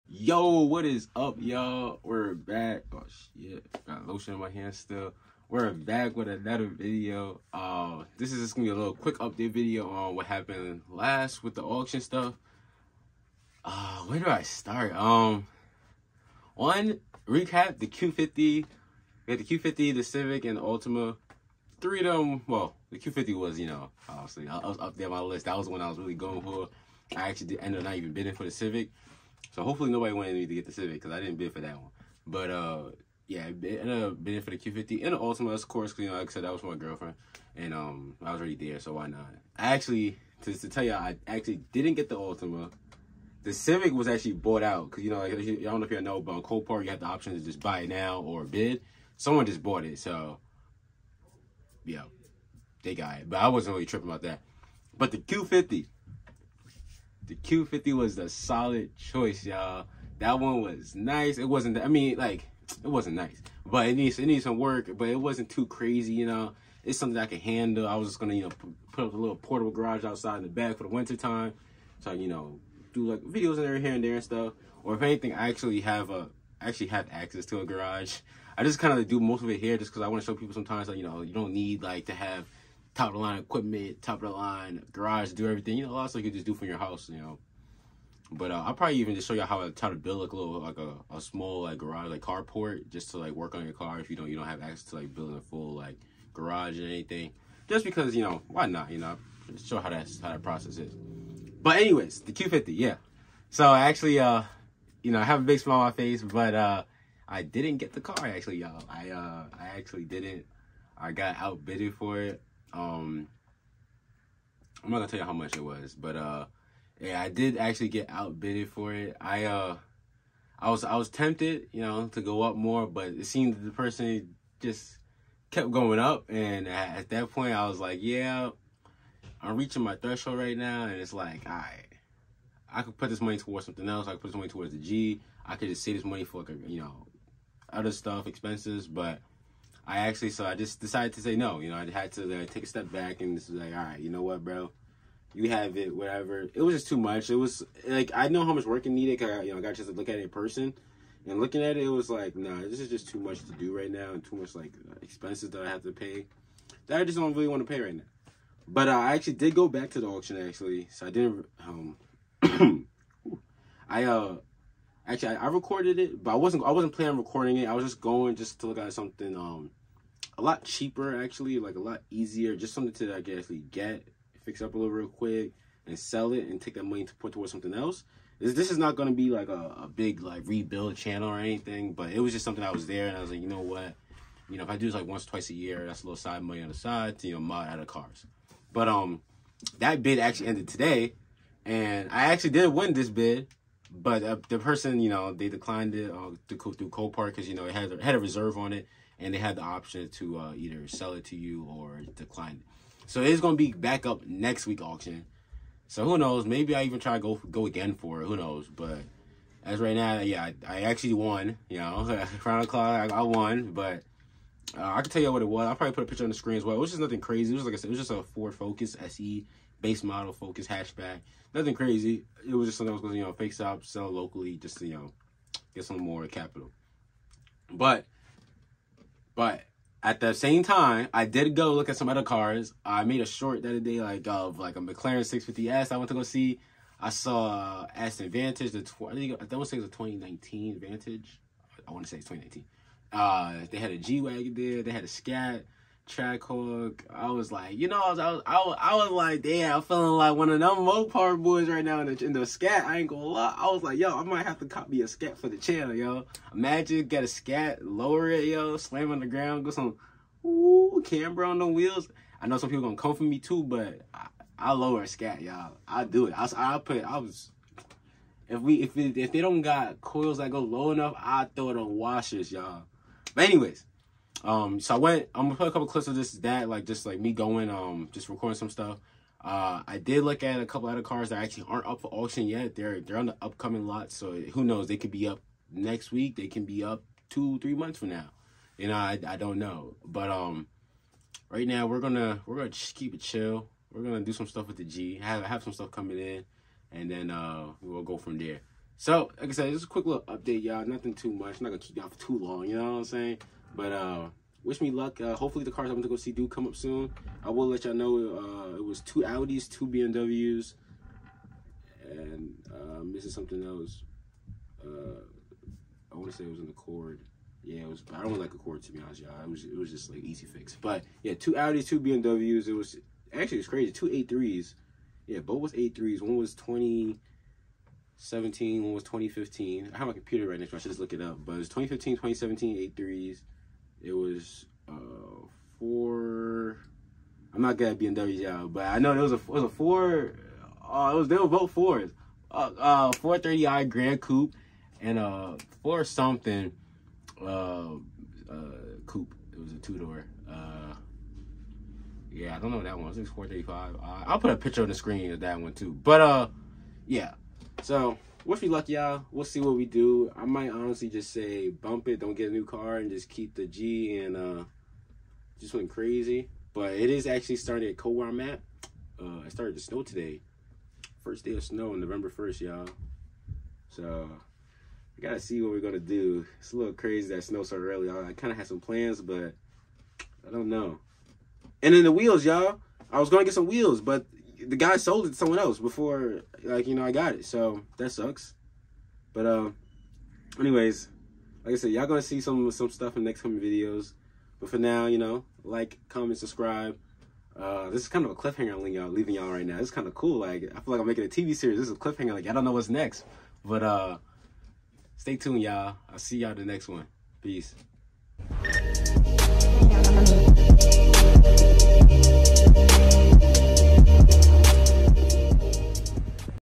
Results Of The Auction | New Car?
Welcome to the channel, last video I went shopping for a new car at the auctions and found some nice options for the channel. Today I find out if I even got any of the nice options.